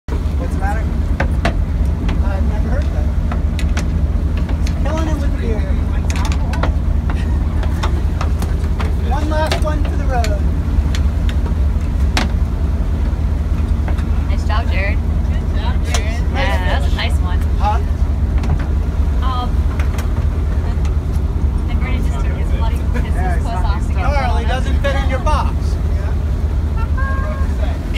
Killing Wahoo with Beer
This 57 pound Wahoo, caught of the shores of Virginia Beach, used beer to quickly kill the wahoo so it didn't slash and hurt anyone. Those teeth can be killer. Cheers🍻